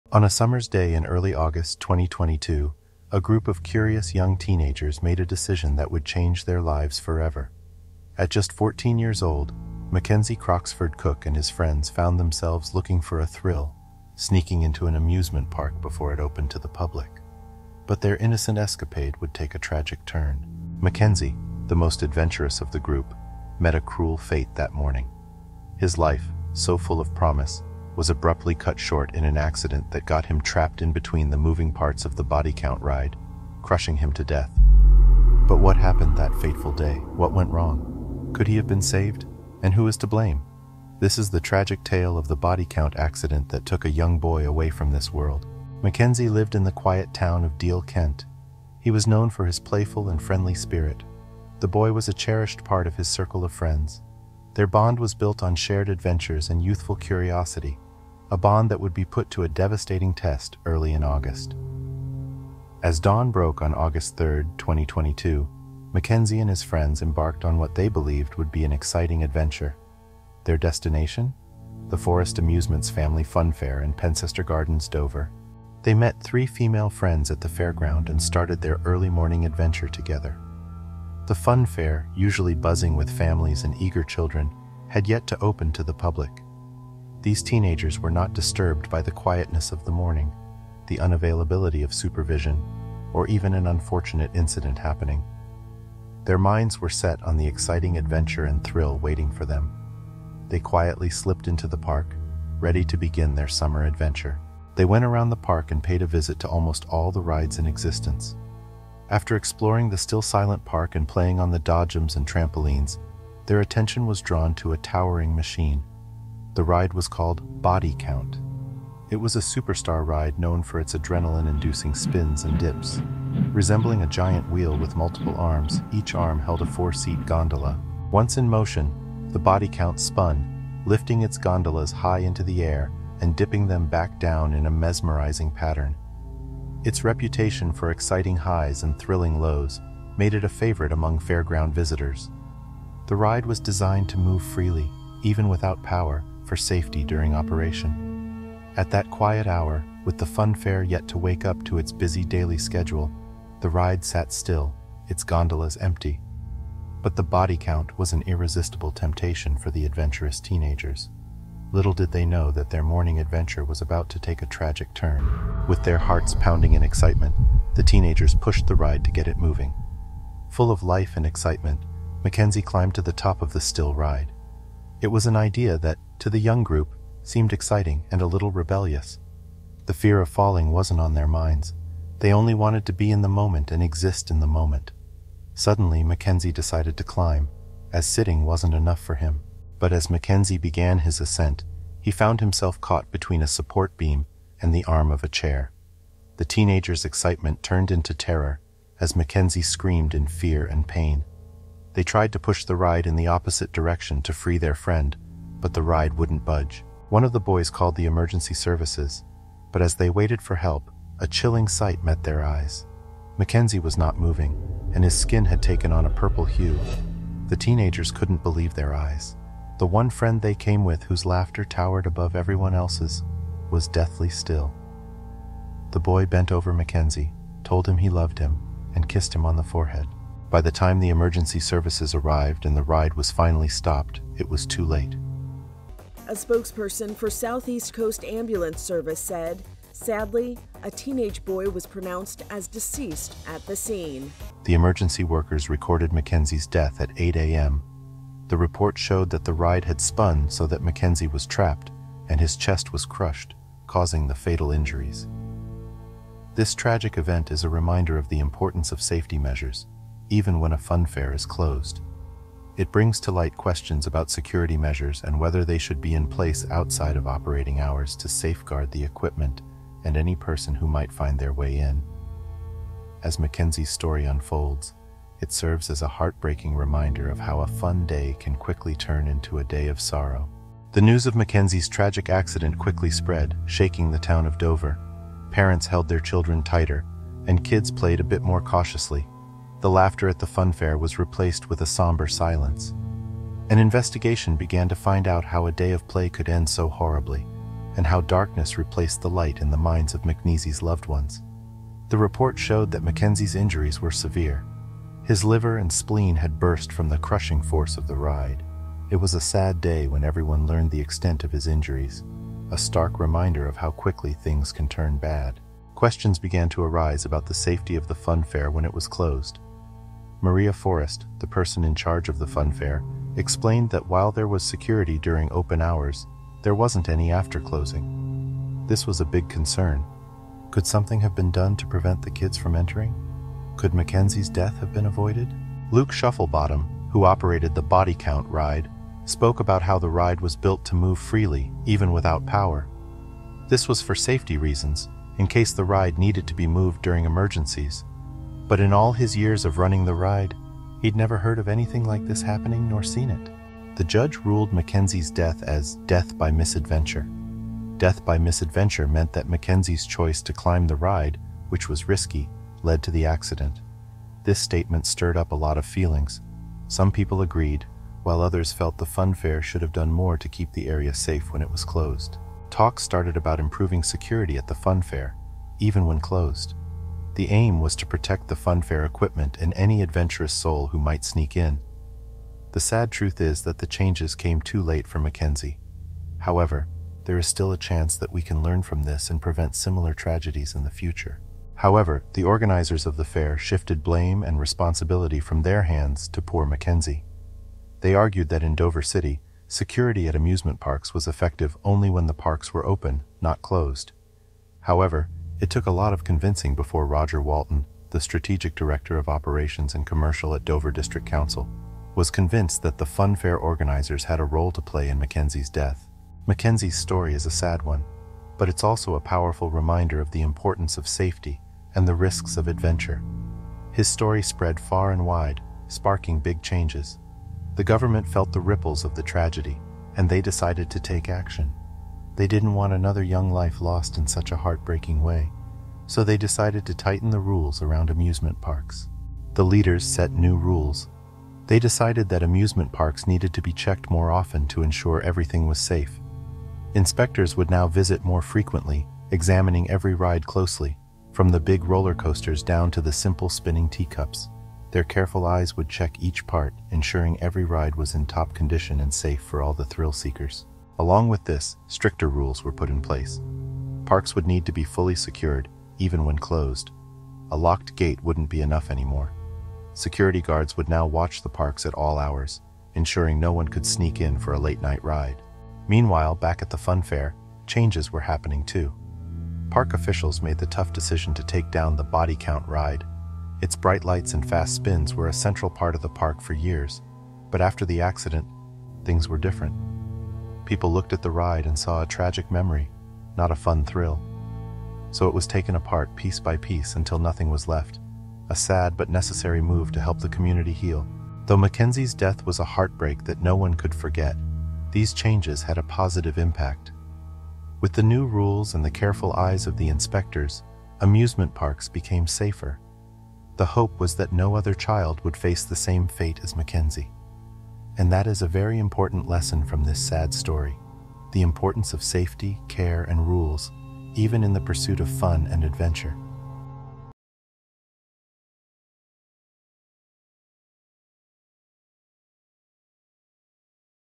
Body Count Ride BRUTALLY KILLED Young Boy
On a summer's day in early August 2022, a group of curious young teenagers made a decision that would change their lives forever.

At just 14 years old, Mackenzie Croxford-Cook and his friends found themselves looking for a thrill, sneaking into an amusement park before it opened to the public. But their innocent escapade would take a tragic turn.

Mackenzie, the most adventurous of the group, met a cruel fate that morning. His life, so full of promise, was abruptly cut short in an accident that got him trapped in between the moving parts of the Body Count ride, crushing him to death.

But what happened that fateful day? What went wrong? Could he have been saved? And who is to blame?

This is the tragic tale of the Bodycount accident that took a young boy away from this world.

🎥 You can expect at least 2 video's per week on our channel